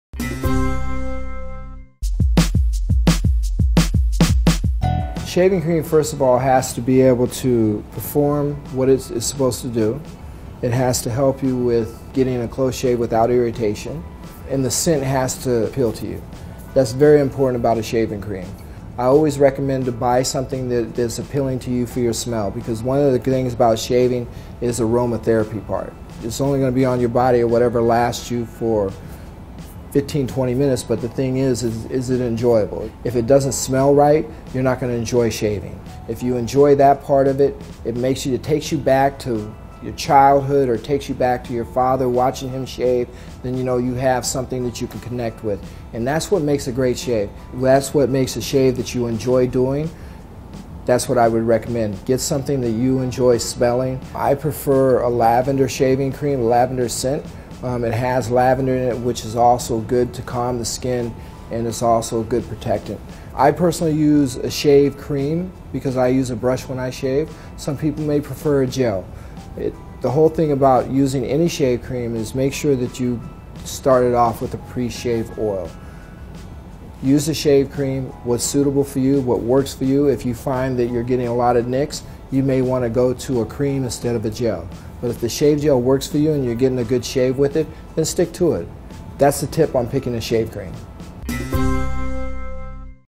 How to Pick a Shaving Cream | Shaving Tips
Shaving cream, first of all has to be able to perform what it's supposed to do. It has to help you with getting in a close shave without irritation and the scent has to appeal to you. That's very important about a shaving cream. I always recommend buying something that's appealing to you for the smell, because one of the things about shaving is the aromatherapy part. It's only going to be on your body, lasts you for 15 or 20 minutes, but the thing is, is it enjoyable?

If it doesn't smell right, you're not going to enjoy shaving. If you enjoy that part of it, it takes you back to your childhood or takes you back watching your father shave, then you know you have something that you can connect with and that's what makes a great shave. You ask what makes a shave that you enjoy doing, that's what I would recommend. Get something that you enjoy smelling. I prefer a lavender shaving cream, lavender scent. It has lavender, which is also good to calm the skin, and it's also good protectant.

I personally use a shave cream, because I use a brush when I shave. Some people may prefer a gel. The whole thing about using any shave cream is make sure that you start it off with a preshave oil. Use a shave cream that's suitable for you, that works for you. If you find that you're getting a lot of nicks you may want to go to a cream instead of a gel. But if the shave gel works for you and you're getting a good shave with it, then stick to it. That's the tip on picking a shave cream.